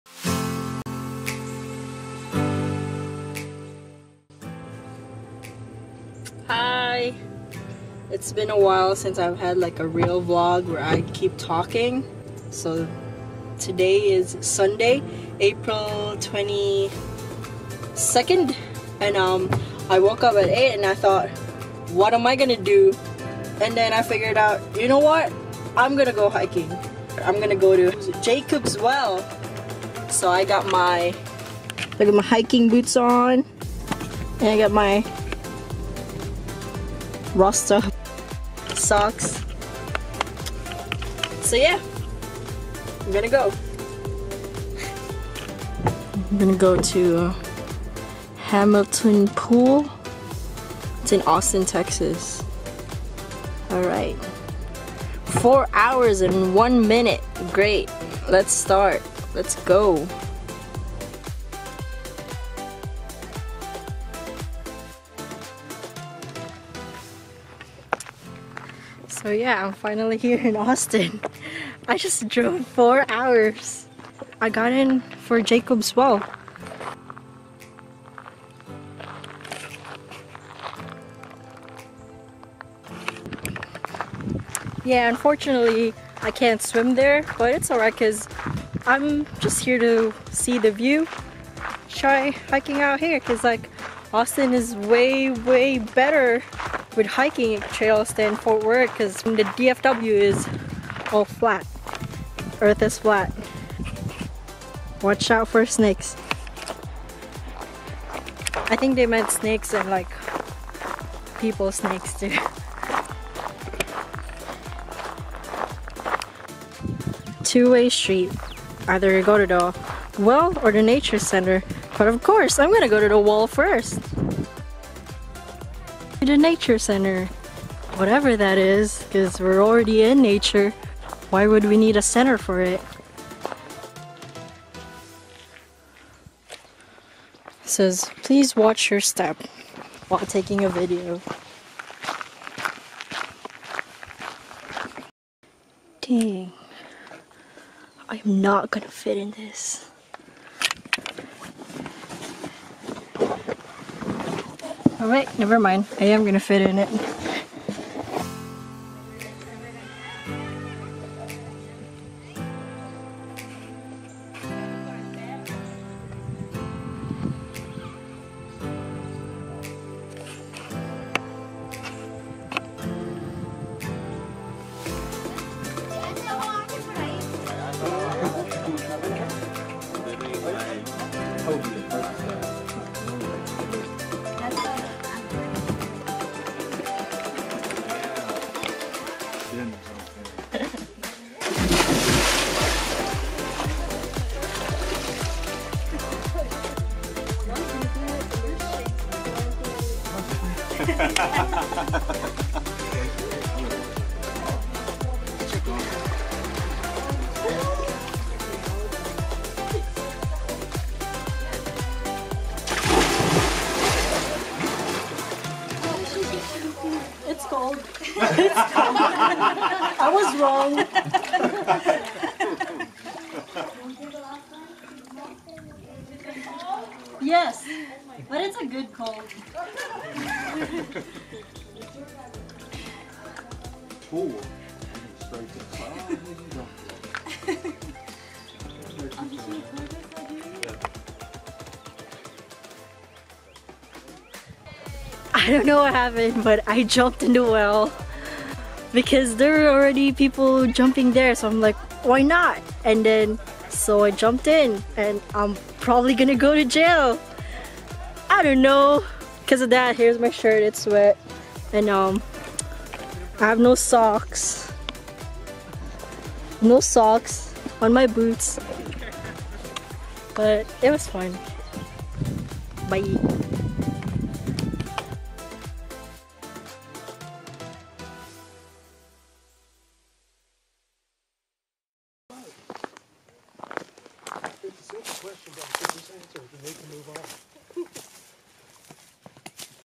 JACOB'S WELL, AUSTIN TEXAS
So last saturday 04/21/18, I slept the whole day so when I woke up the next day, I was like " I NEED TO GO DO SOMETHING!" so I had this brilliant idea to go hiking! Jacob's Well has been on my must to go place eversince I got here to this country and I'm so glad I finally got that off my list! The place was beautiful! No Regrets! Will definitely go back to see more of Austin,Texas.

Hope ya'll enjoy my video.

 - SHNEN SARMI
Check my facebook page @BisayaSaGawas
Created with Wondershare Filmora
Background Music from Wondershare FIlmora
Other Background Music from: